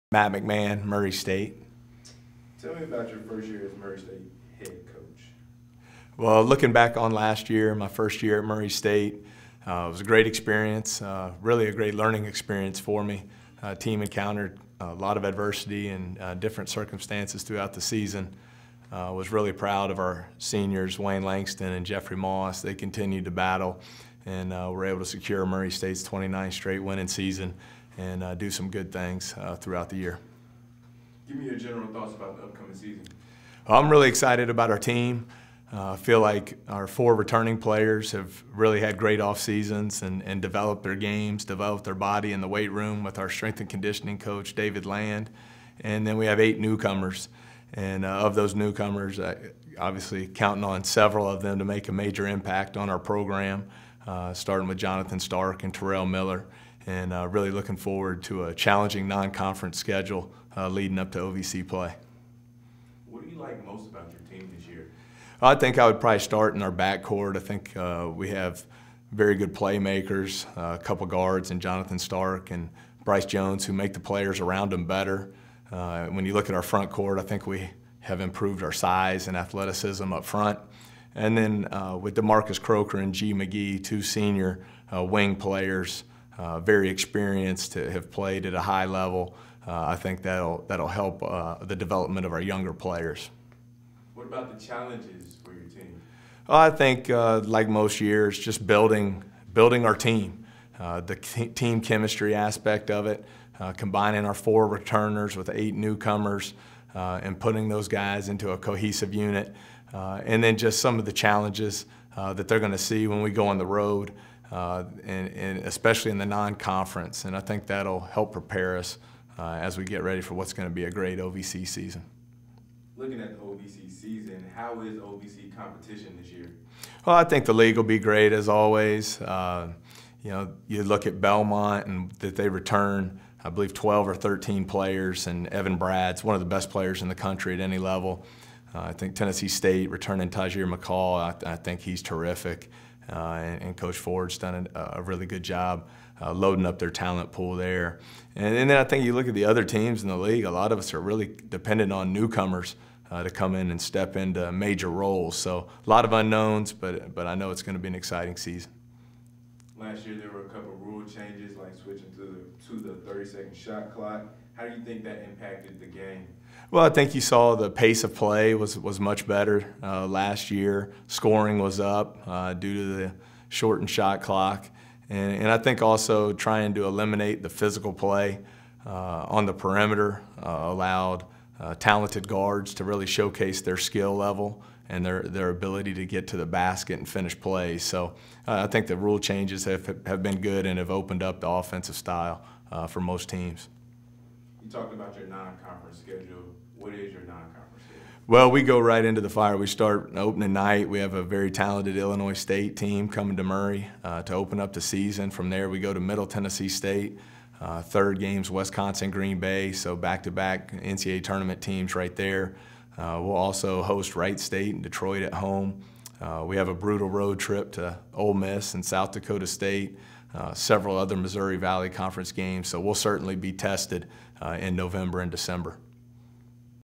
Matt McMahon, Murray State (2016 OVC Basketball Media Day)
Murray State men's basketball coach Matt McMahon talked about the upcoming season at the 2016 OVC Basketball Media Day.